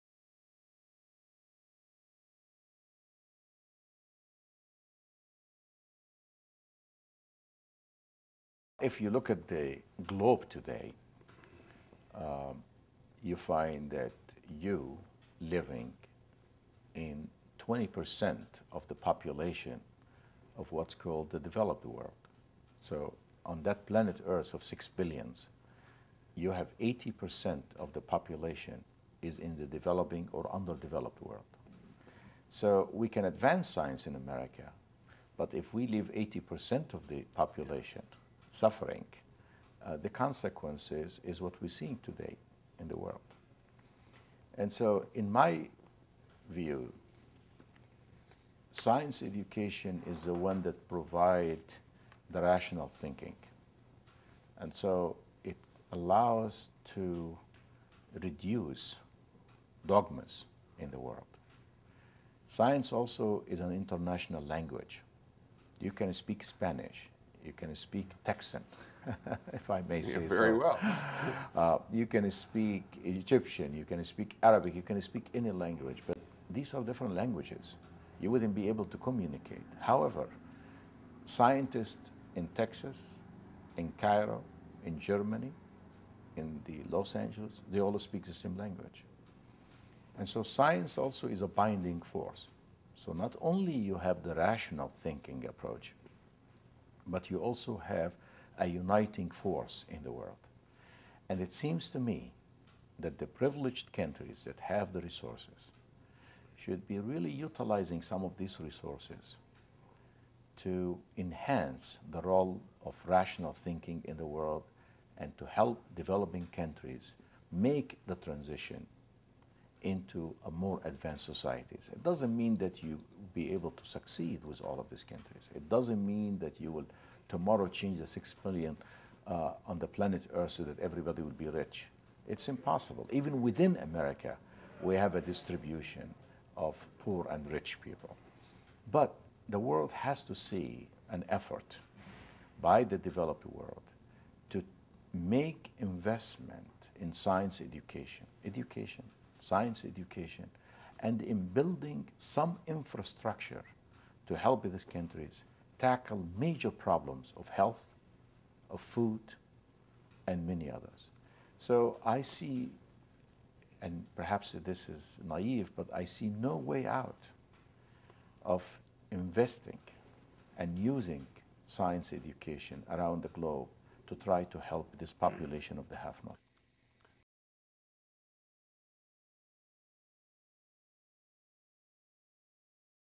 Ahmed Zewail - Science Education
Nobel laureate Ahmed Zewail and the importance of science education in every part of the world.